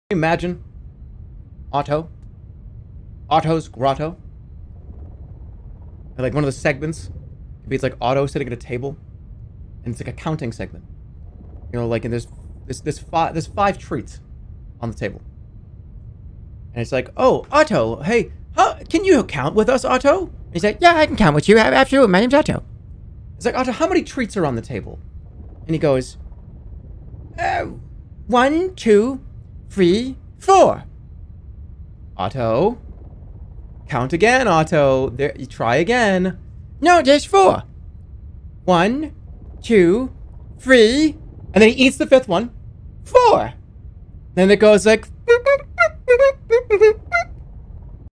Otto's Grotto (Jerma Animated)
@2ndJerma and Otto conceptualise their new TV show.